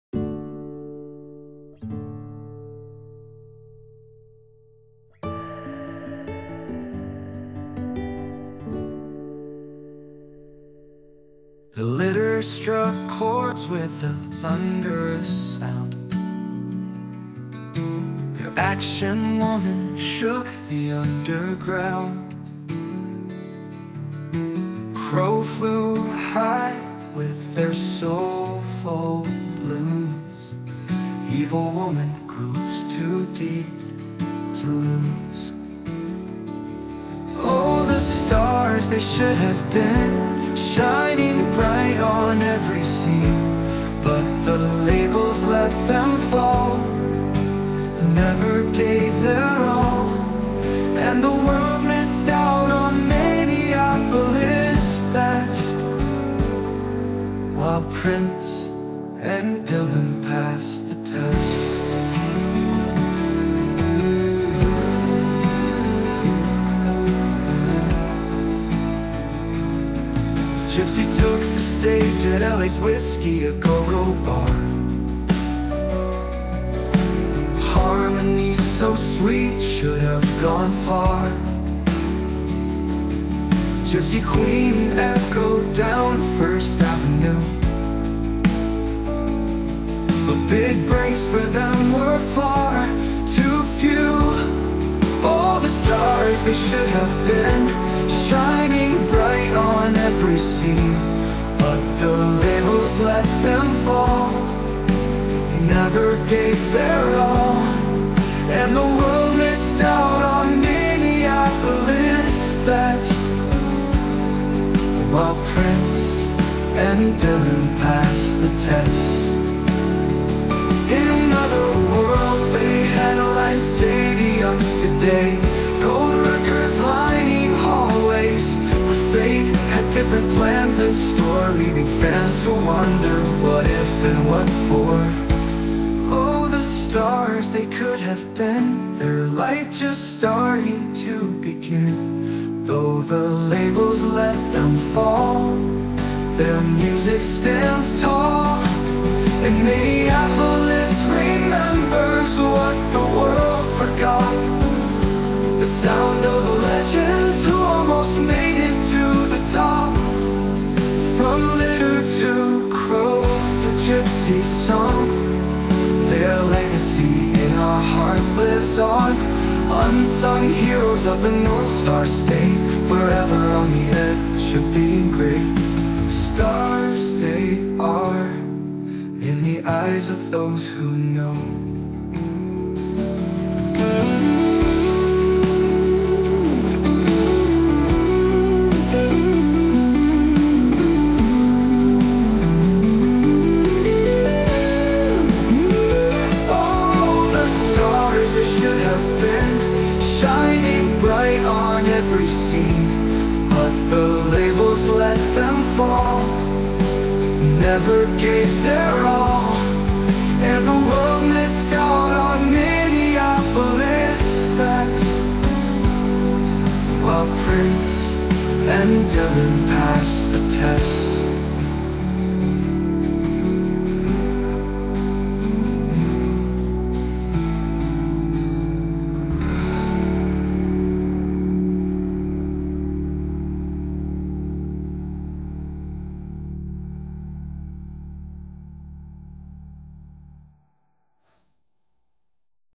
Crow-Gypsy-The Litter-The Stars They Should Have Been-MMHC Legend Series
Video tribute to  Minnesota Artists
Crow
Gypsy
and The Litter

All received national recording  contracts and put out spectacular LP's
but  never got the label support to really hit the big time.